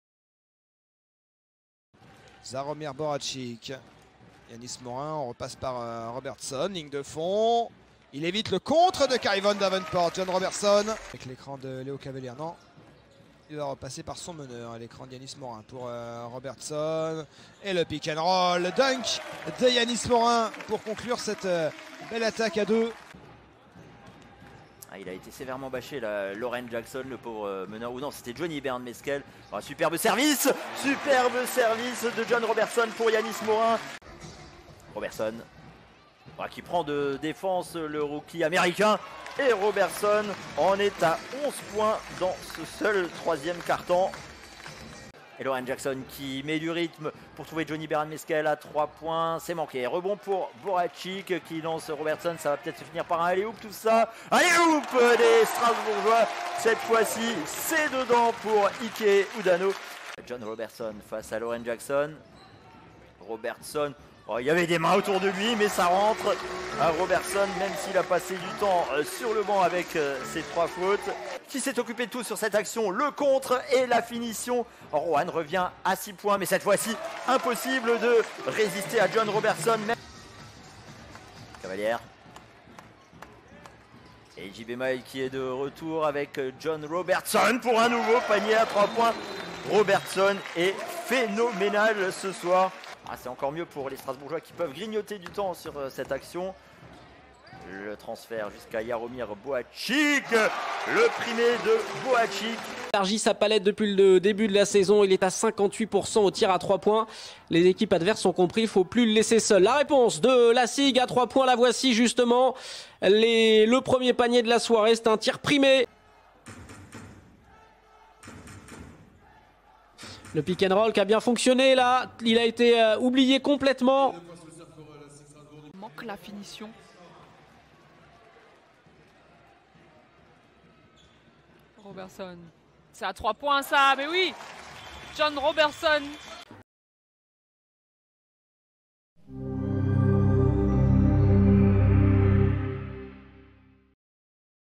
John Roberson : ses highlights de Novembre 2021
Découvrez une sélection des highlights du SIGman du mois en Betclic ELITE lors du mois de novembre 2021